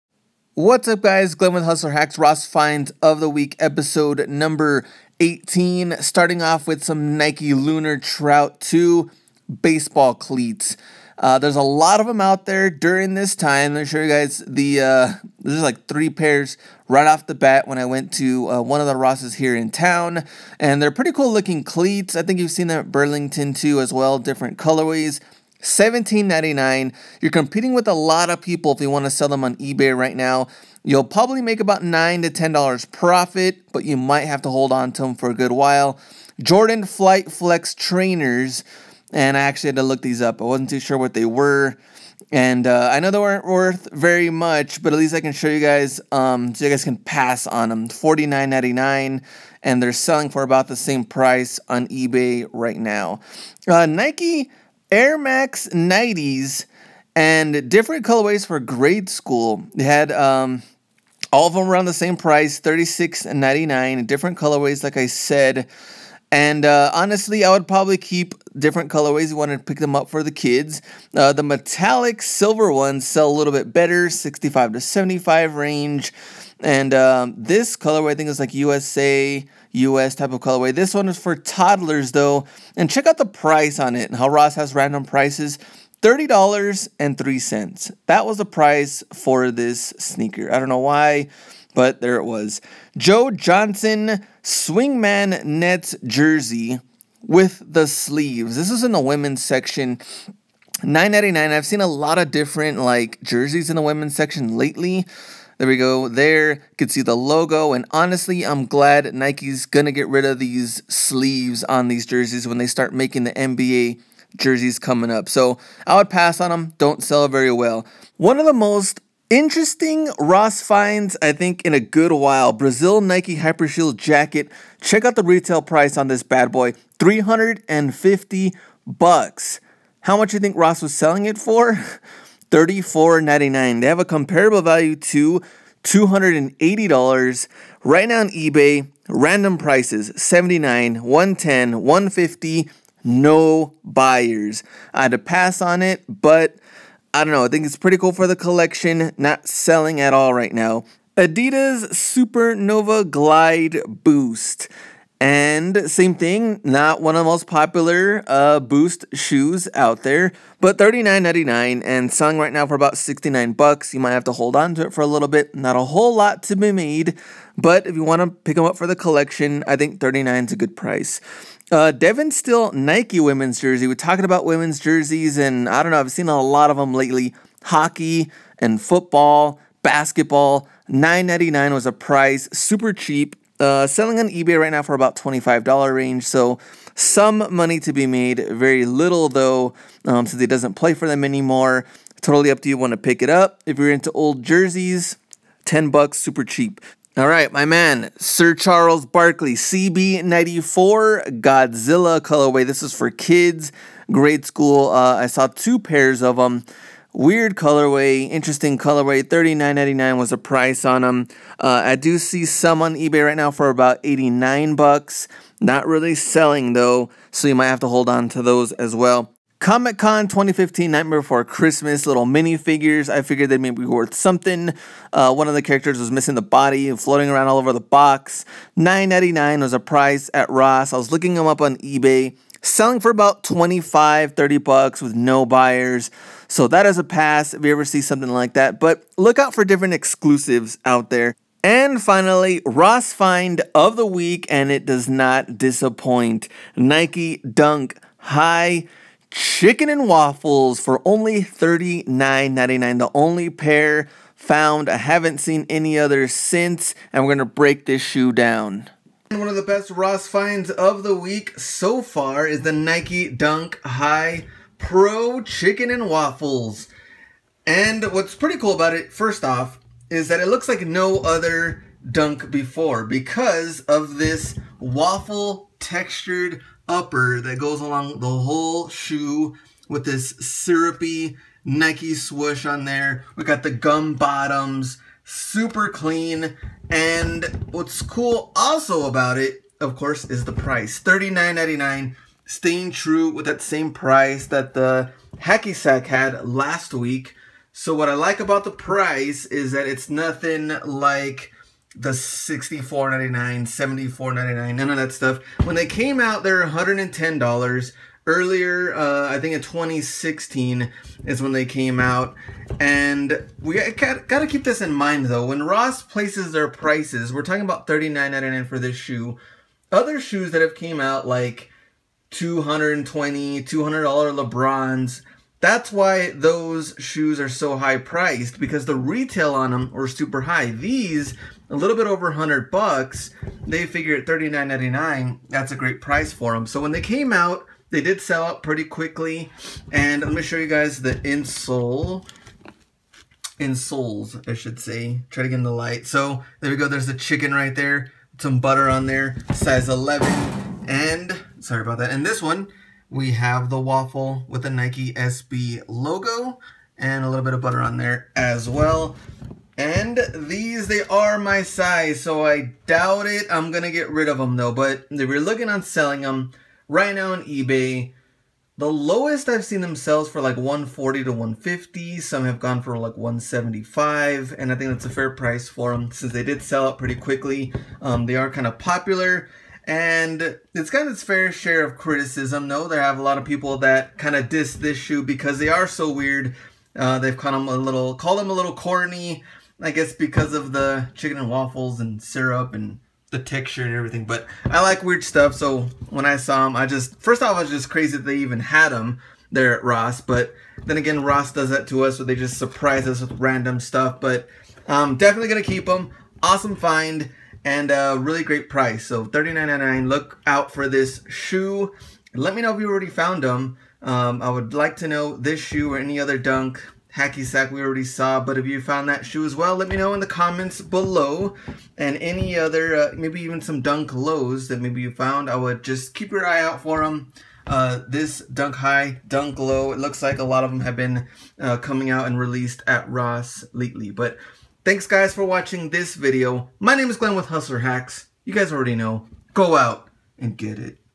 Ep. 18- Ross Finds o/t Week (2/2/17) - Nike SB Dunk Chicken & Waffles
Finds include Nike SB Dunk High Chicken and Waffles and more! Some of these finds are great to pick up to Resell, some are great for the collection and others are just simple items to pass on.